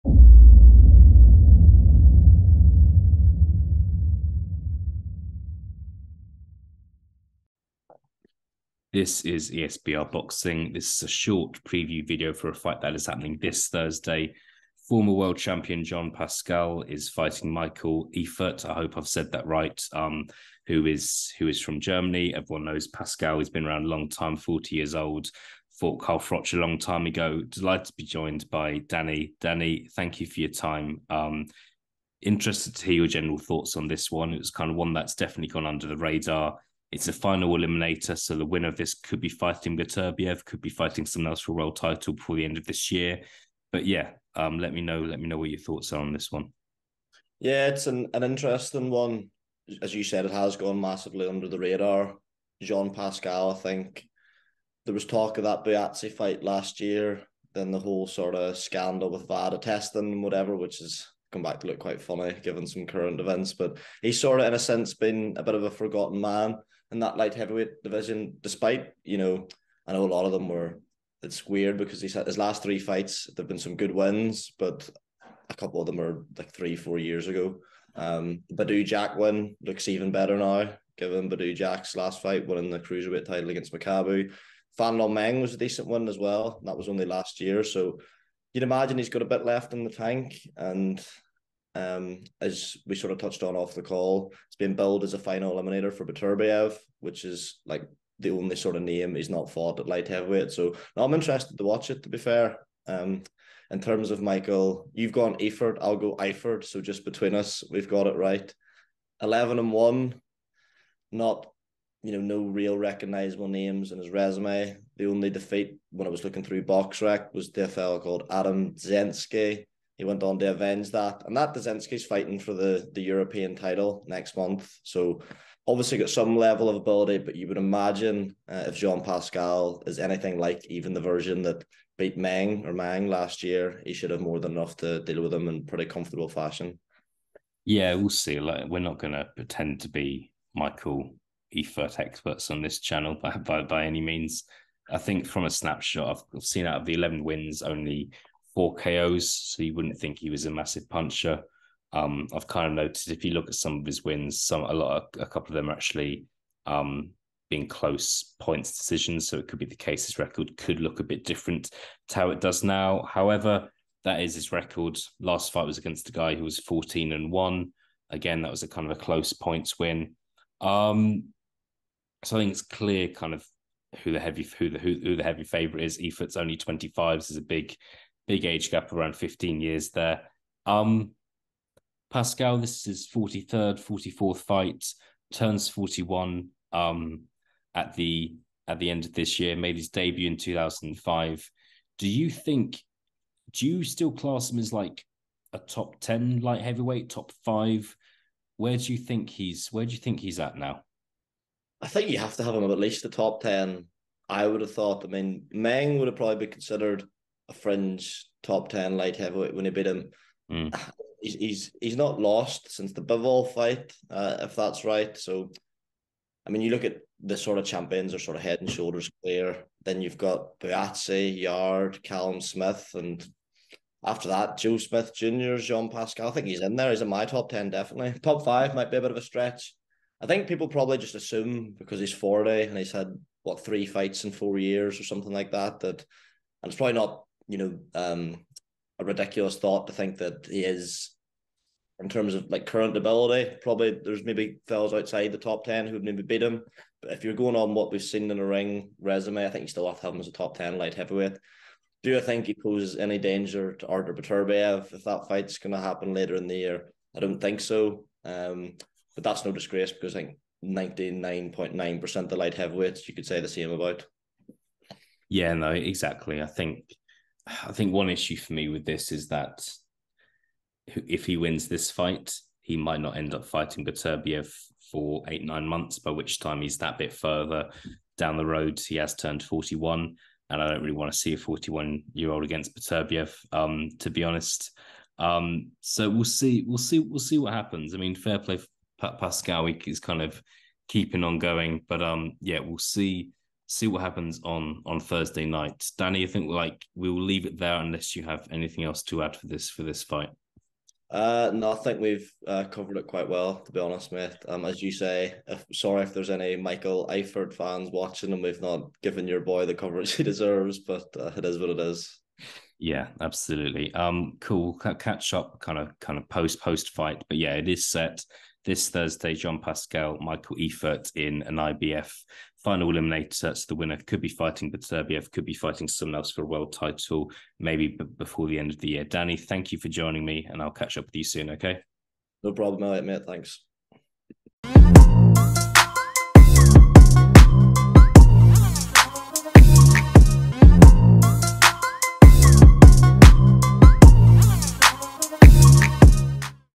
JEAN PASCAL vs MICHAEL EIFERT PREVIEW - Has Pascal Still Got It?
Danny and Eliot discuss this Thursday's final eliminator for the IBF world light heavyweight title between Jean Pascal and Michael Eifert ⚠️ FIND ESBR ON SOCIAL MEDIA⚠️